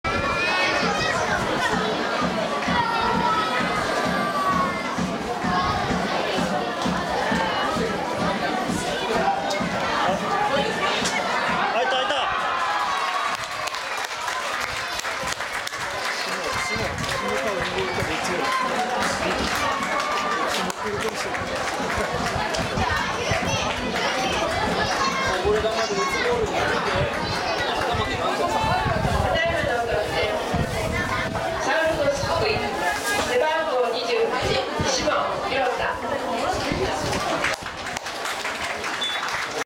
09北信越リーグ サウルコス福井vs上田ジェンシャン 霜ゴール【HD】
２００９年５月３１日敦賀市陸上競技場　北信越リーグ第７節サウルコス福井vs上田ジェンシャン　後半３５分サウルコス福井：霜良太